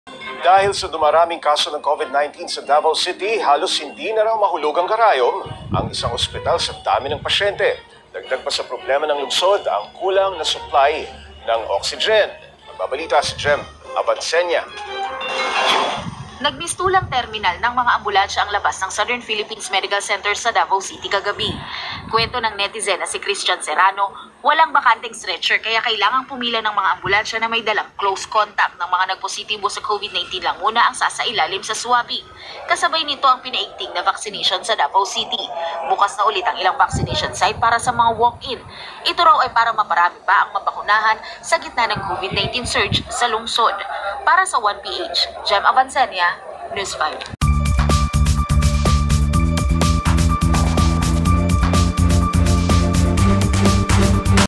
Mga ambulansya, nakapila sa labas ng ospital sa Davao City
OneBalitaPilipinas | Nagmistulang terminal ng ambulansya ang labas ng Southern Philippines Medical Center (SPMC) sa Davao City dahil sa dami ng COVID-19 patients na dinadala sa ospital. | via Gem Avanceña